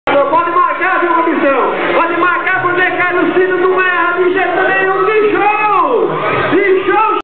Carlos Filho armada positiva na Festa do Laço em Caracol (MS)
O 21º Encontro do Laço da Federação do MS aconteceu das 26 a 28 de março de 2010, no Clube do Laço Retiro do Caracol com transmissão ao vivo pela Cidade FM 106,3 e Integração FM 104,9.